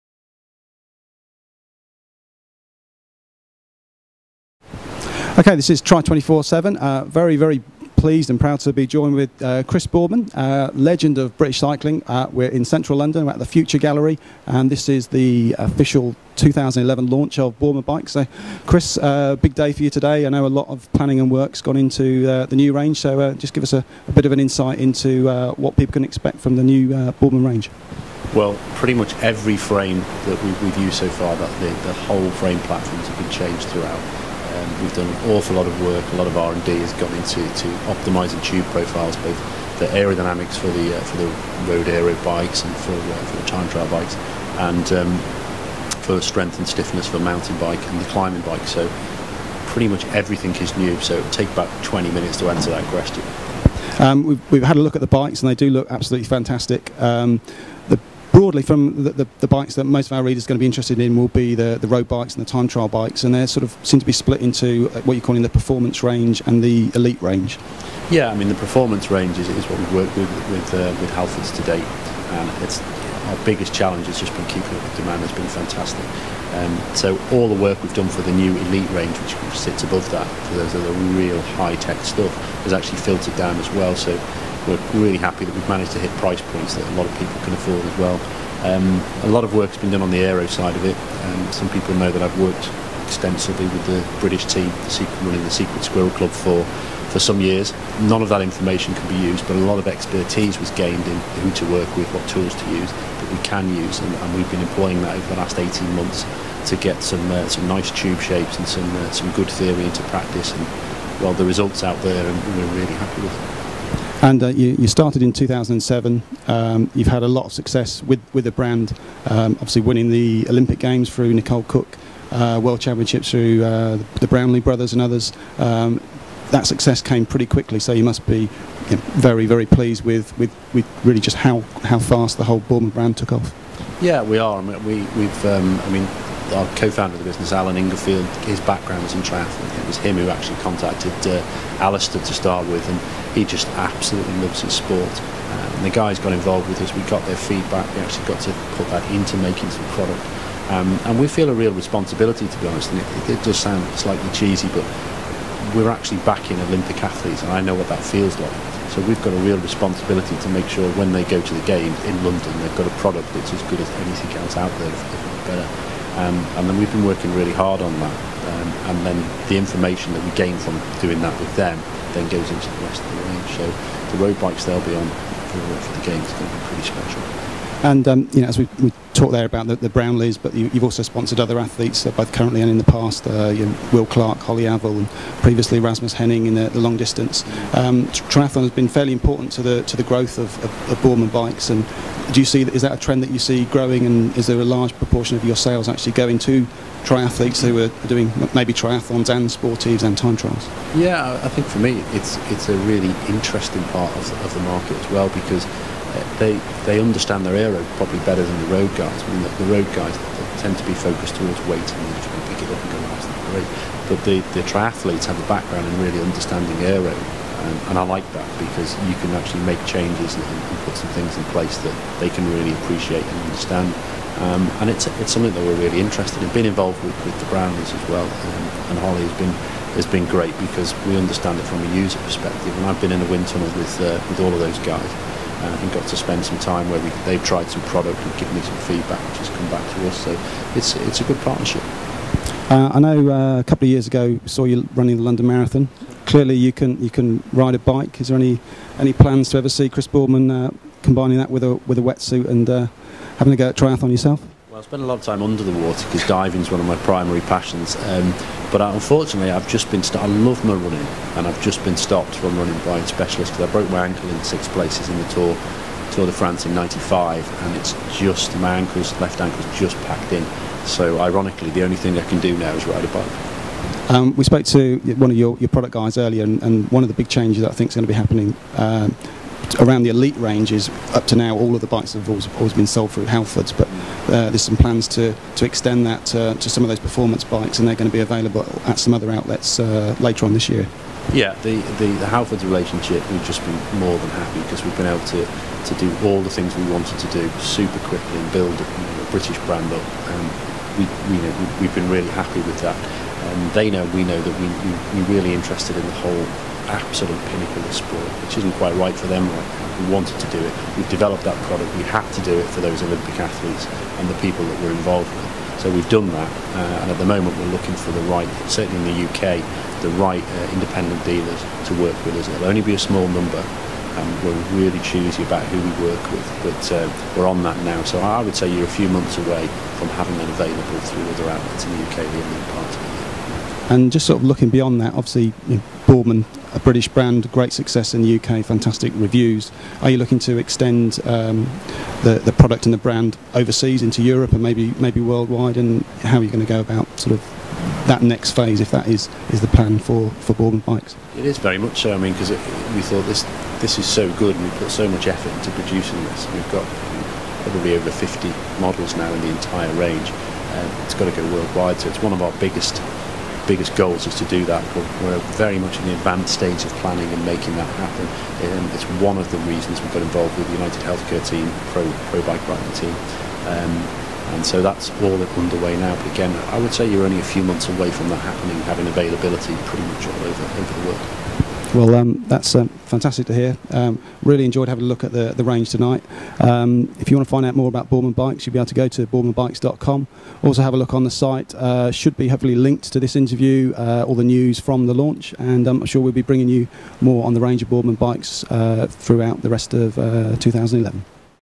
Boardman Bikes Launch 2011 Chris Boardman
Tri247 Editor John Levison interviews Chris Boardman at the launch of the 2011 Boardman Bikes range at the Future Gallery, Central London